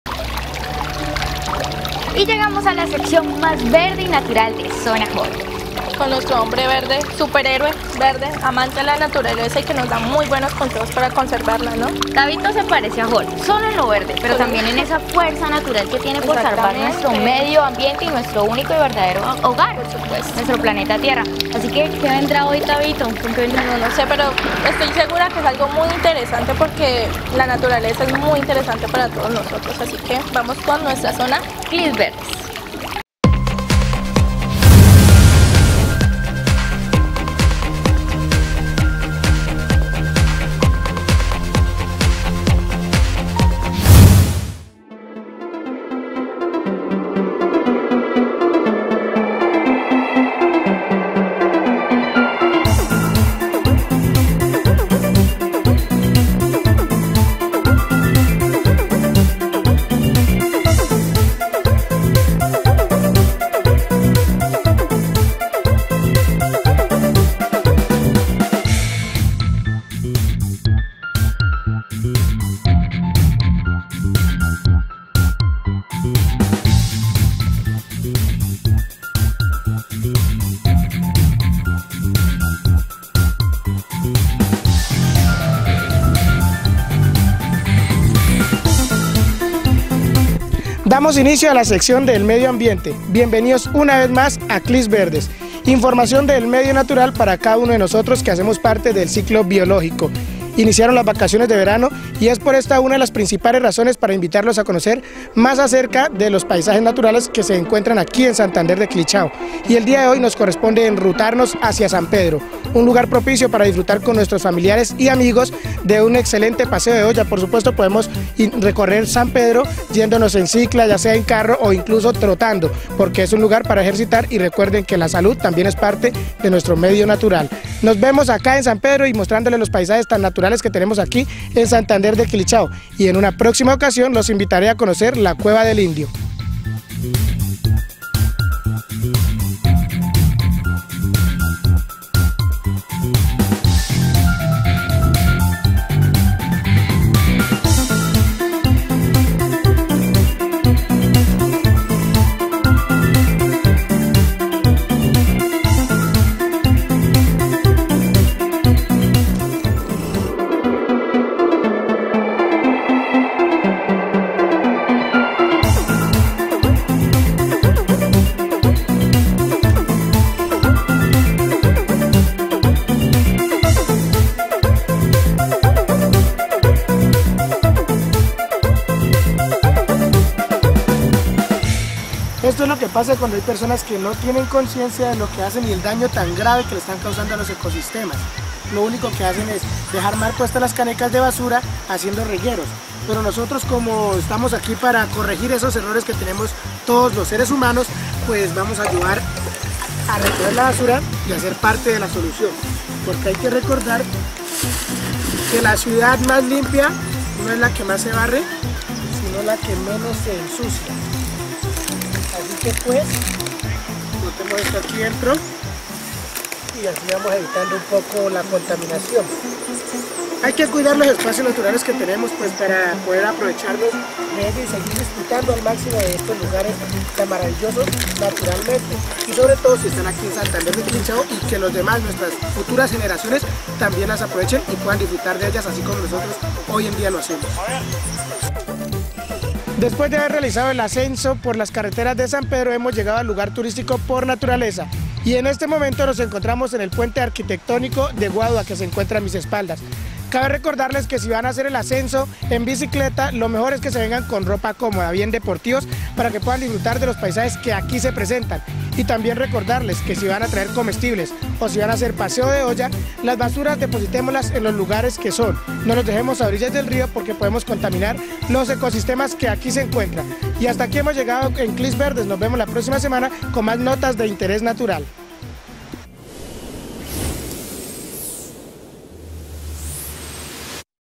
San Pedro, Santander de Quilichao
Zona Joven te invita a conocer San Pedro, Un Gran lugar para divertirse, hacer deporte  y pasarla junto a los tuyos.